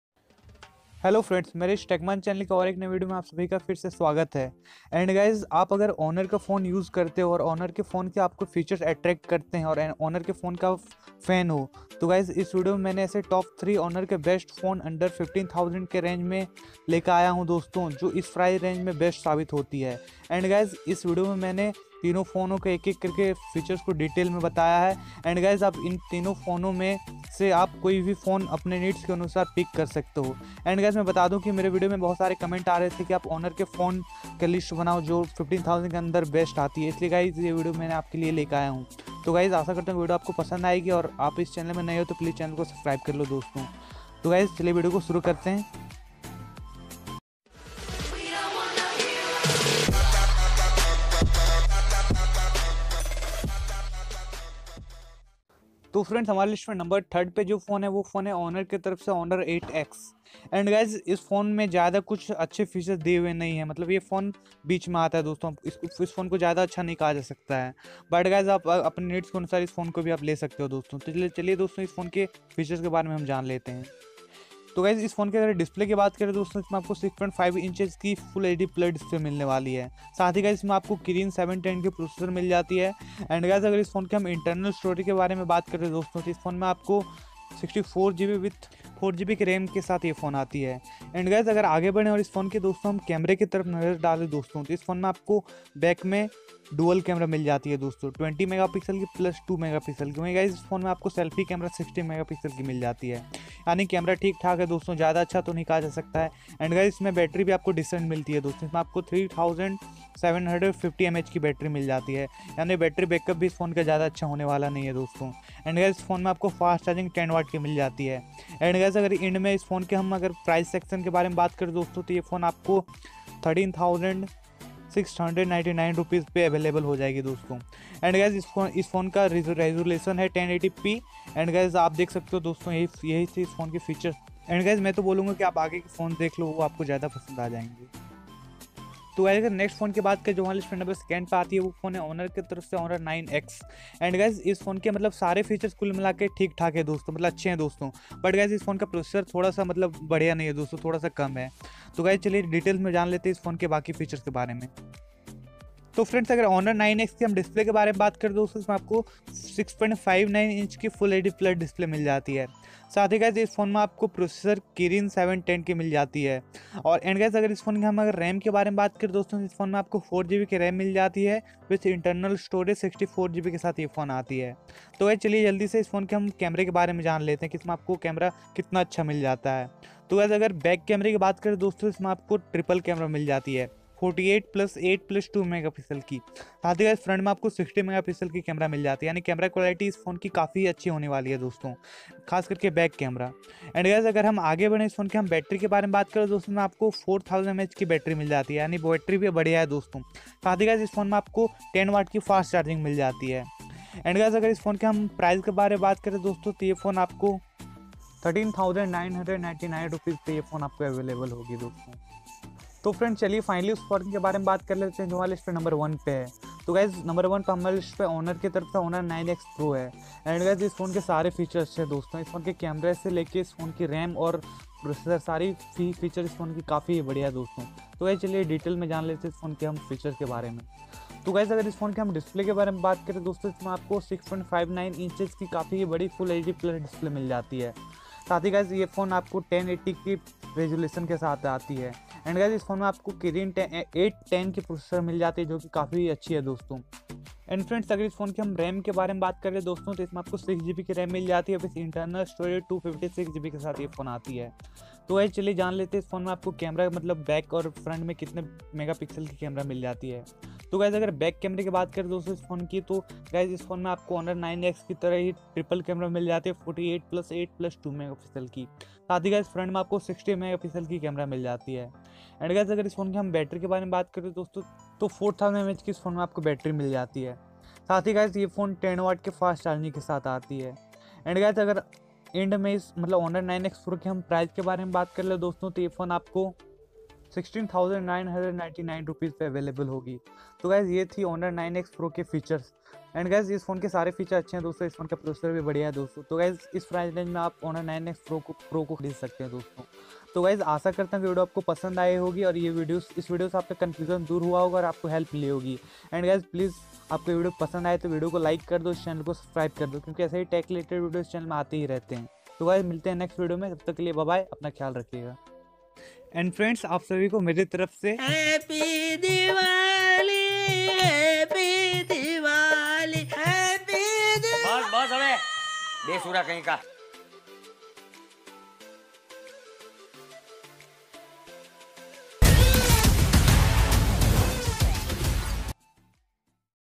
Top 5 Best Honour Smartphone Under 15000 in November 2020
Hello friends in this video we discuss about Top 5 Best Honour smart phone Under 15,000

SOURCE: Xiaomi, Huawei, Argus World Plus